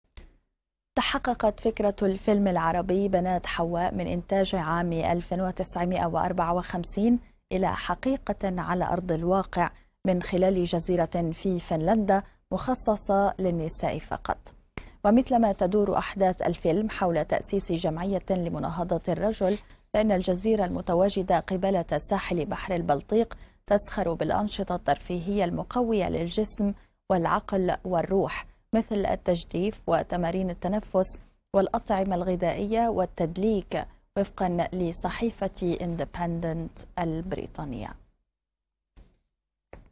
جزيرة للنساء فقط في فنلندا
تحققت فكرةُ الفيلم العربي بنات_حواء من إنتاج عام 1954 إلى حقيقةٍ على أرض الواضع، من خلالِ جزيرةٍ في فنلندا، مخصصة للنساء فقط.

ومثلما تدورُ أحداثُ الفيلم حول تأسيسِ جمعيةٍ لمناهضة الرجل، فإن الجزيرة المتواجدة قبالةَ ساحلِ بحر البلطيق، مسخرة لخدمة عضوات مؤسسة نسائية تدعى "سوبر شي" أو "سوبر هي"، والتي تزخرُ بالأنشطة الترفيهية المقوية للجسم والعقل والروح، مثل التجديف، وتمارين التنفس، والأطعمة الغذائية، والتدليك، وفقا لصحيفة "إندبندنت" البريطانية.

وترجع فكرة إنشاء الجزيرة، إلى سيدة الأعمال سوبر شي، التي اشترتها في سبتمبر/أيلول 2017.

وحصلت "ماتيسيا للاستشارات" في عام 2015، وهي شركة استشارية تابعة لكريستينا روث، على المركز السابع في قائمة مجلة "فوربس" الأسرع نموا في قائمة الشركات المملوكة للنساء، وكانت إيراداتها في ذلك الوقت تبلغ 45 مليون دولار، لتقرر صاحبتها أن تبيعها بعد عام، وتطلق مؤسسة تدعى "سوبر شي" على الجزيرة، التي توفر الكثير من أشكال الترفيه للعضوات، بمقابل مادي يصل إلى 8000 دولار.

وعن فكرة "سوبر هي"، توضح روث إن مجتمع "سوبر هي" يتألف من نساء مستقلات ماديا ونفسيا، ويسعين إلى أن يكونوا أفضل نسخة من أنفسهن.